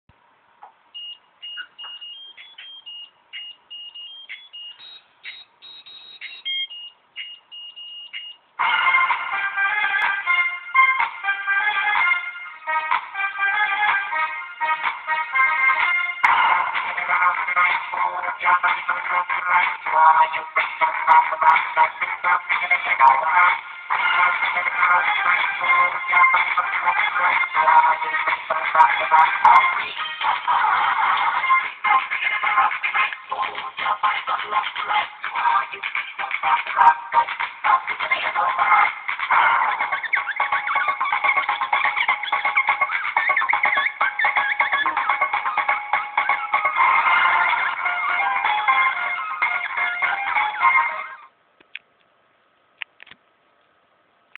Jades famous eyebrow dance
So you want to see my darling daughters eyebrows move, boy! they cant half strutt their stuff. to the famous advery of the eyebrow dance